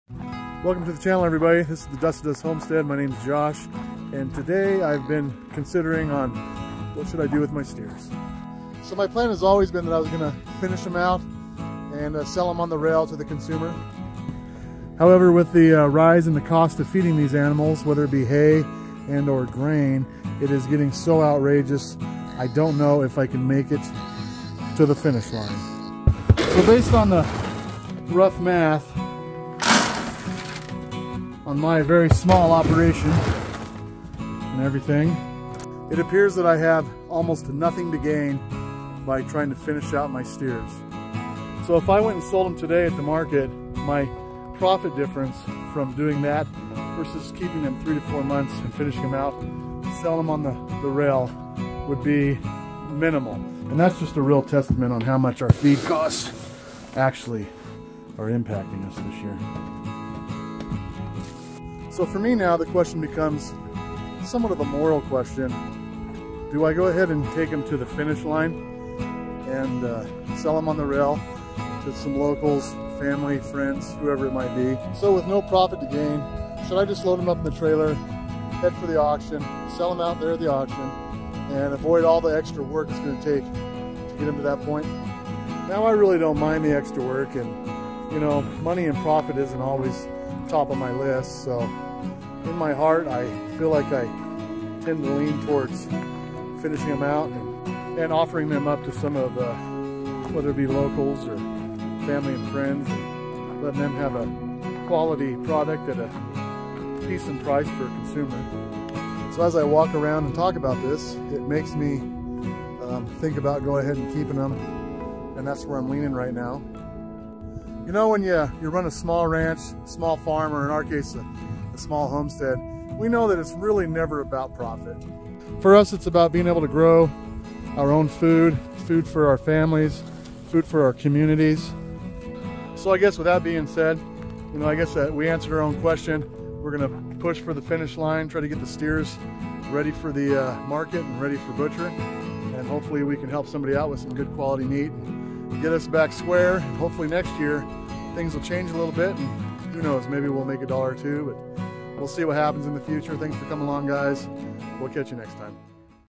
Big dilemma. To sell, or not to sell my steers
trying to decide if finishing our steers is worth it